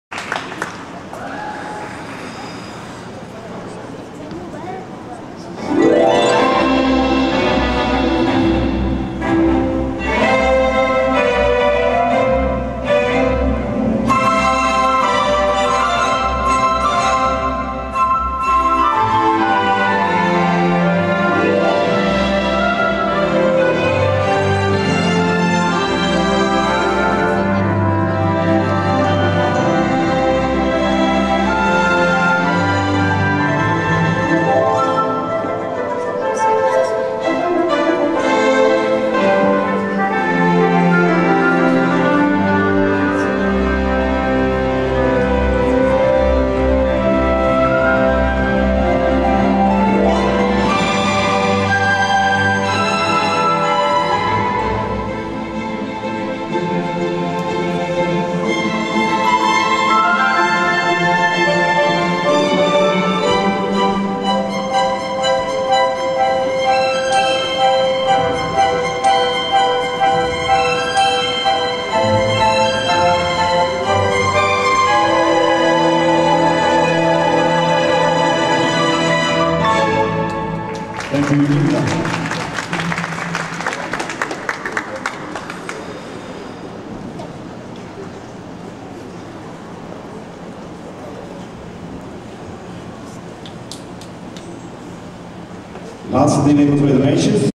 Indra schaatsvoorstelling
Indra op het ijs bij schaatsclub Pirouette op 1 juni 2008